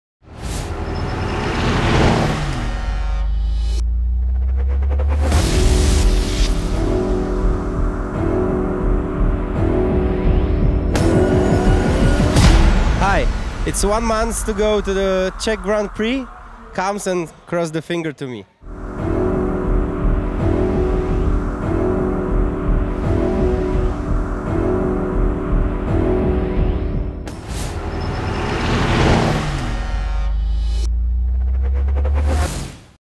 Most is coming - 4 weeks to go!
As soon as the ETRC is back on track from its summer break, things will be taking off really fast: Only 1 week after the ETRC Round 5 at Hungaroring the Czech Truck Prix - representing ETRC Round 6 - will be staged in Most. A race especially the current overall leader Adam Lacko is looking forward to!